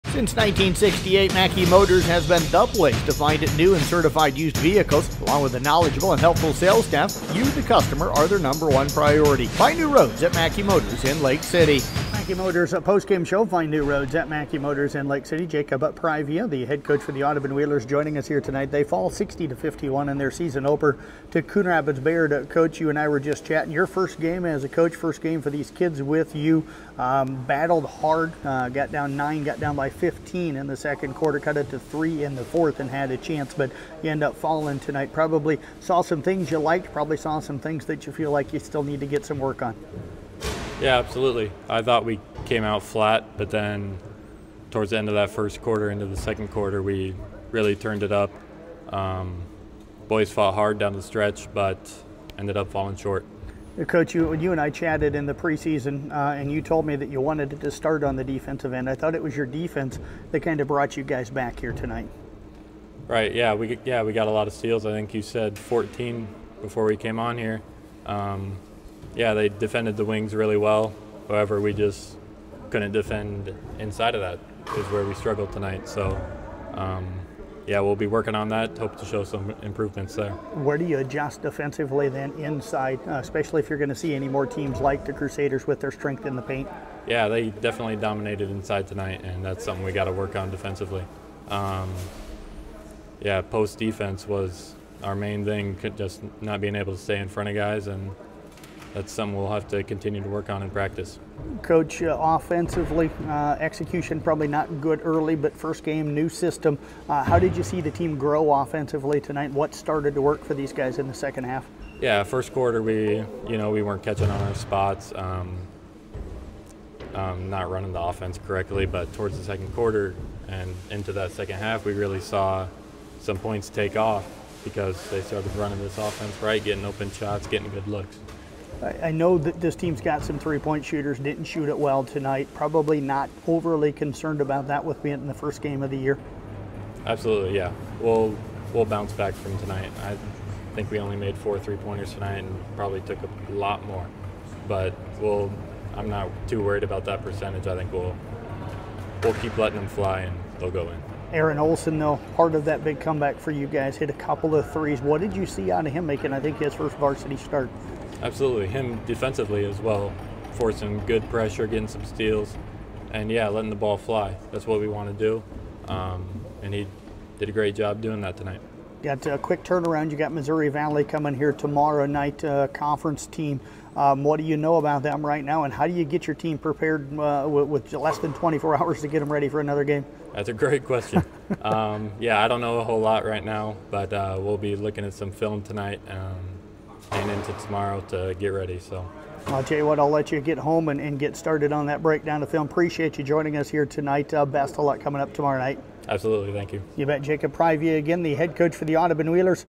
Carroll Broadcasting Sports Network interview w Audubon Head Coach Jacob Privia after CRB 11-28-22
Wheelers battled back but fell 60-51 to CRB